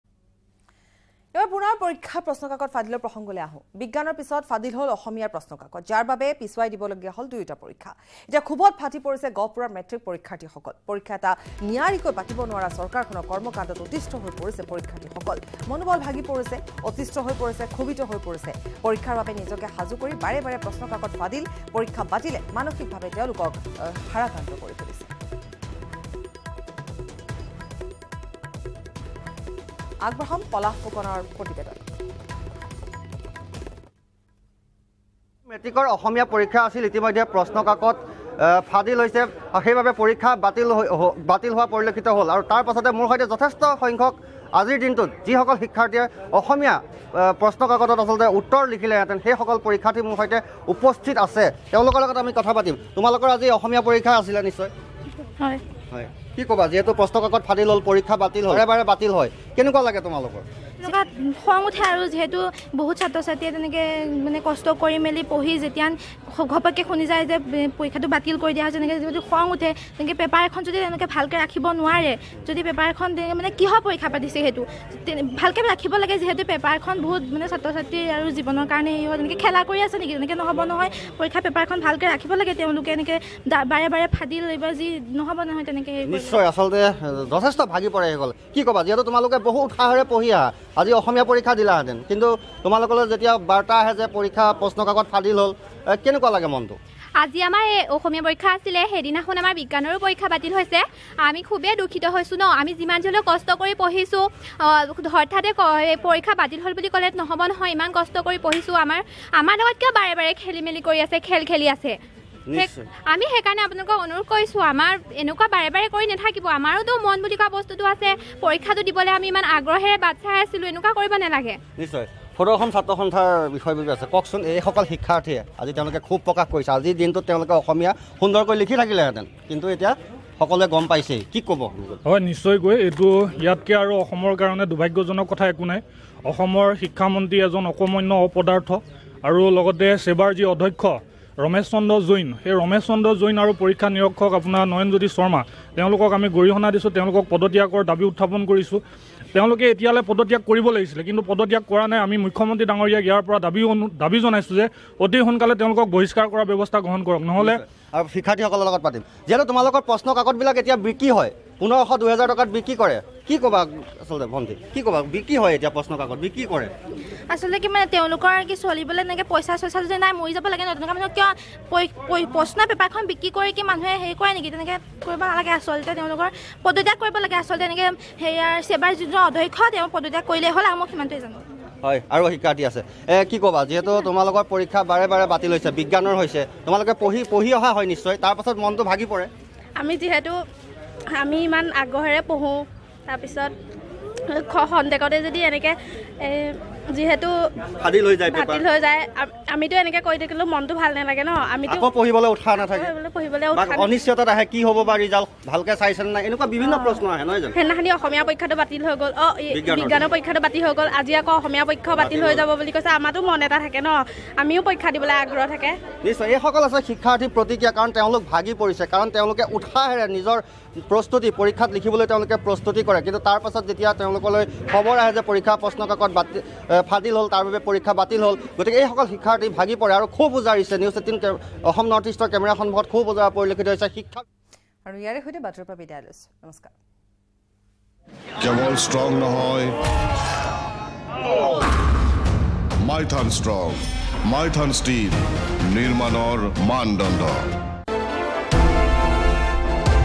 HSLC Candidate Reaction : ক্ষোভত ফাটি পৰিছে গহপুৰৰ মেট্ৰিক পৰীক্ষাৰ্থী সকল | SEBA News
HSLC Candidate Reaction : ক্ষোভত ফাটি পৰিছে গহপুৰৰ মেট্ৰিক পৰীক্ষাৰ্থী সকল | পৰীক্ষা এটা নিয়াৰিকৈ পাতিব নোৱাৰা চৰকাৰখনৰ কৰ্মকাণ্ডত অতিষ্ঠ হৈছে পৰীক্ষাৰ্থীসকল। মনোবল ভাগি গৈছে তেওঁলোকৰ।